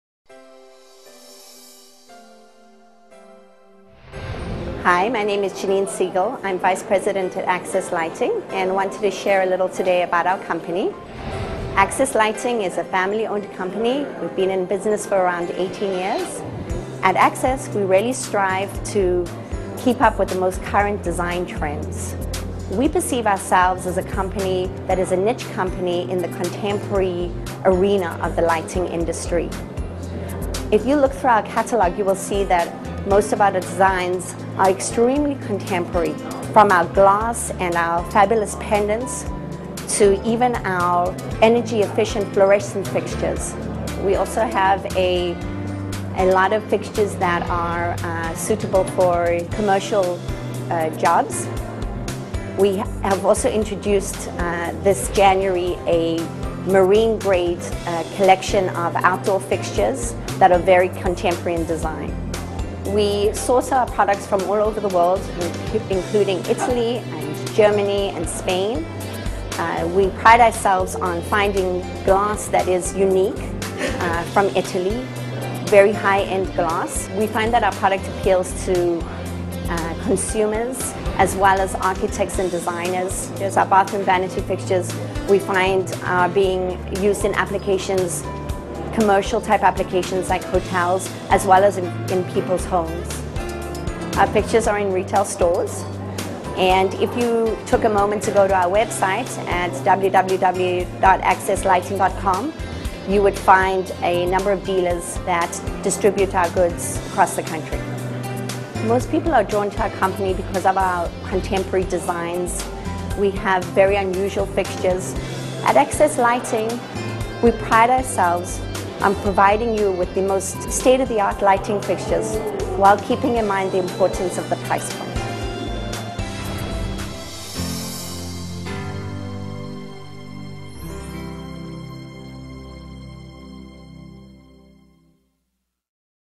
Access Lighting Corporation
Learn more about the wide range of light fixtures available from Access Lighting. Take a look at their amazing pendants, chandeliers and sconces. Access Lighting offers a wide variety of fixtures for any application. May it be for your Bathroom, Bedroom or living room illumination. UpscaleLighting.com offers all the currently available designer lights from Access Lighting.